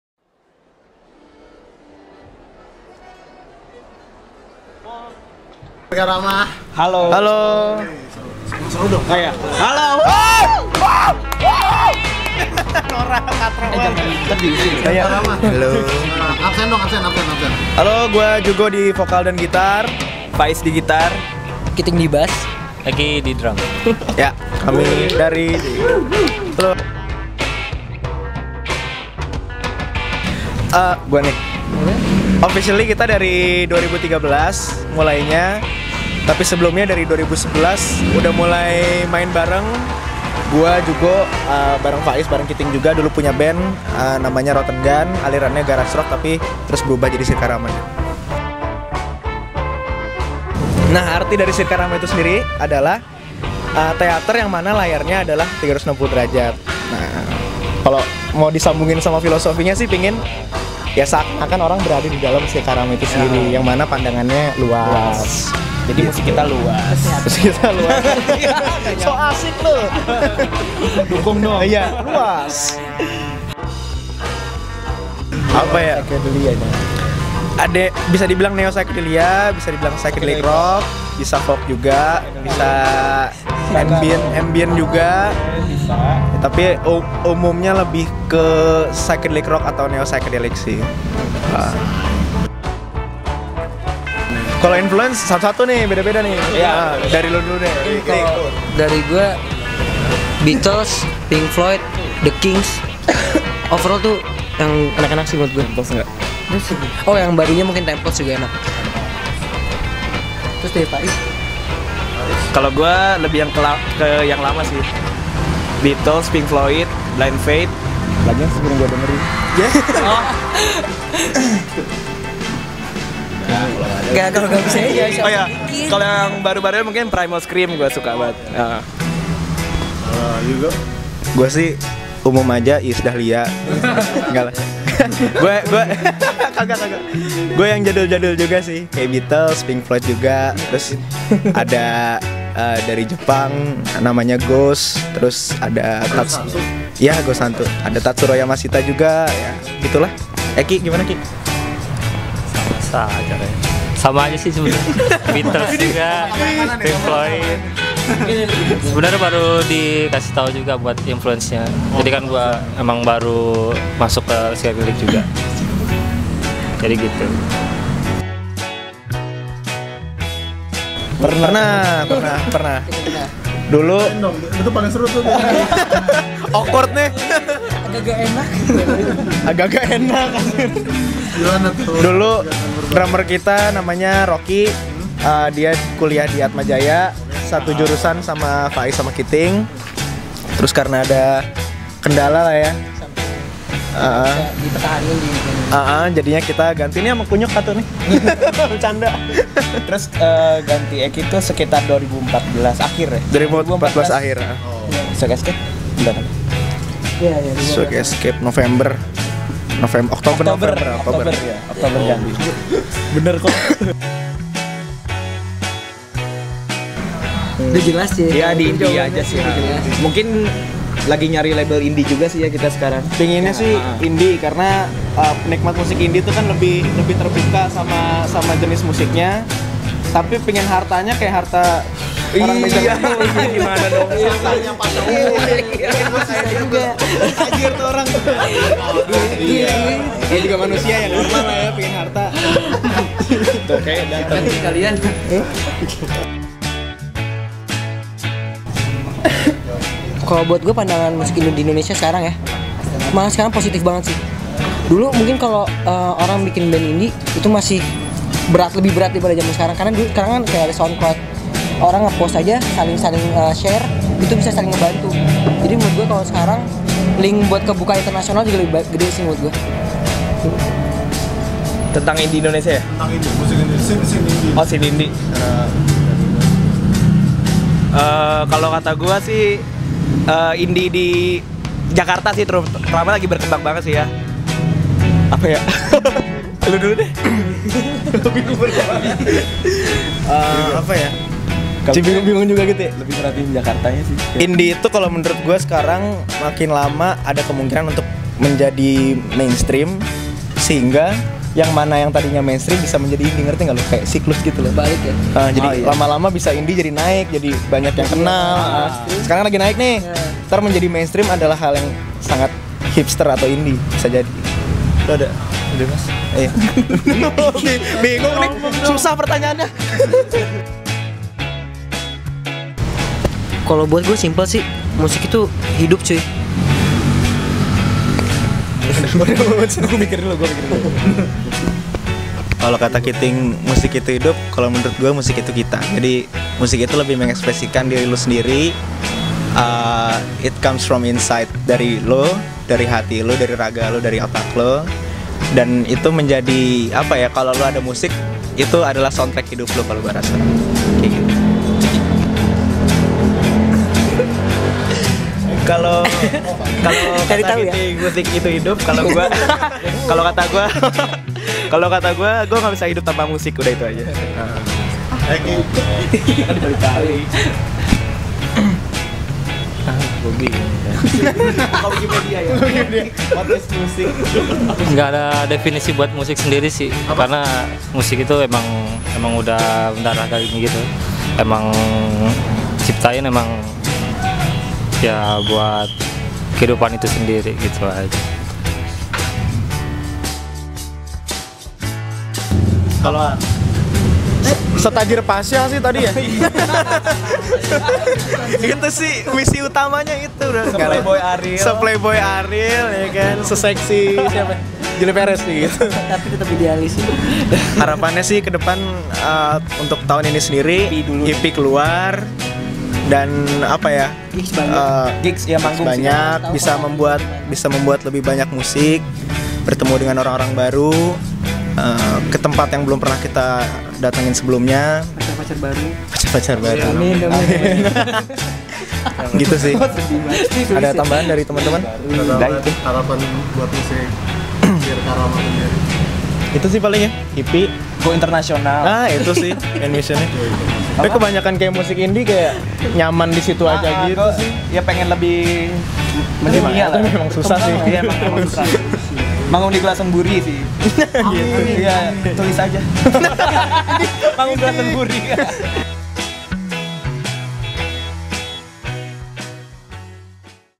Check Sound #4 Circarama
Band psychedelic ini akan berbincang tentang influence hingga apa arti musik menurut mereka. Penasaran sama wawancara kami sama Circarama? Nonton deh yuk!!!

Soundcloud.com/circarama